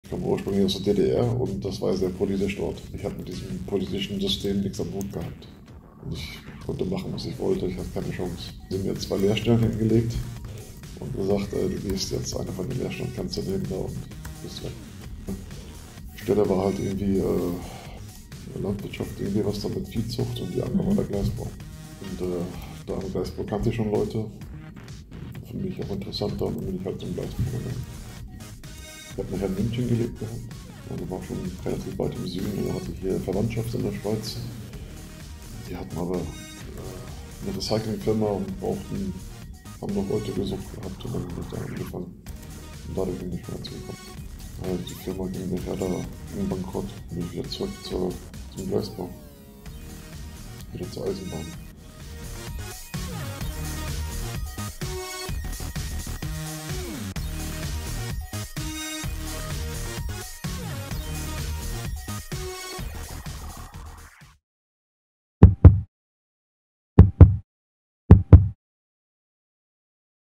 Auf den Spuren von Heiko Moser, Gleisbauer und Baudienstlokführer BLS
Heiko Moser, Gleisbauer und Baudienstlokführer BLS, erzählt aus seinem bewegten Leben.